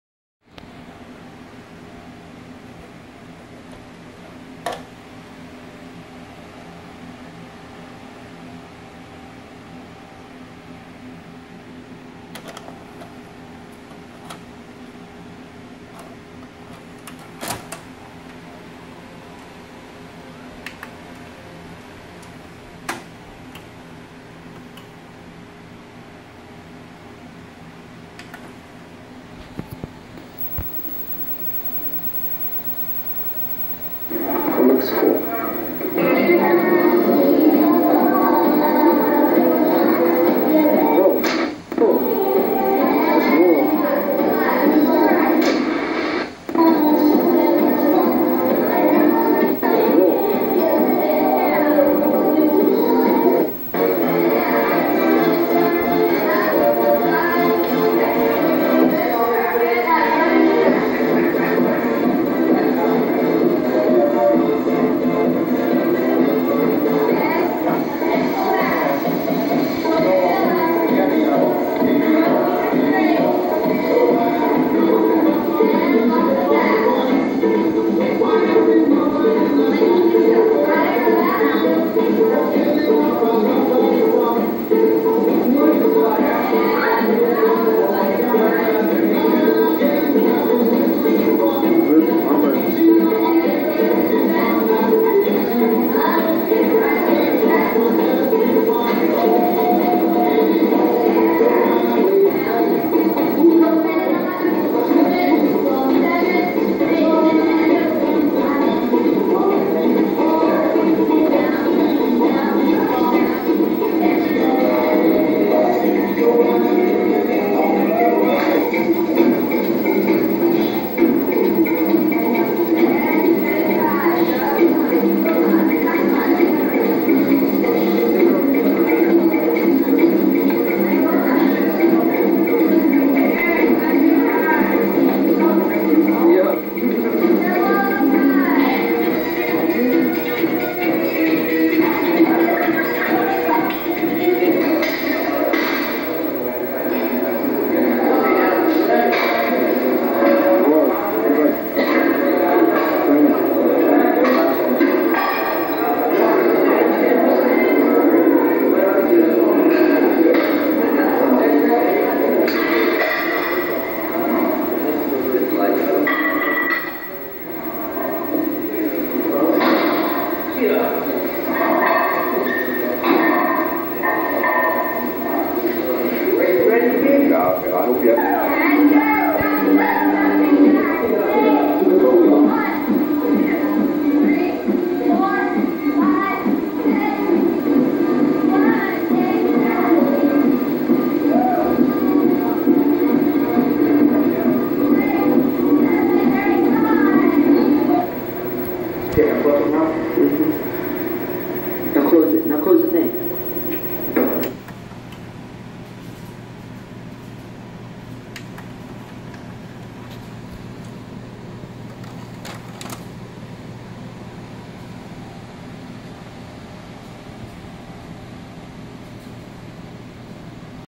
DELTONA HIGH SCHOOL FLORIDA 1992 1993 1994 1995 1996 MISC CAMCORDER
This is just some miscellaneous dance & Weightlifting class footage from back in High school - I found this on a VHS tape that I had from a class I took - I was recording this for class not on my own camcorder - Just thought I would share it. ◇♧☆○□♤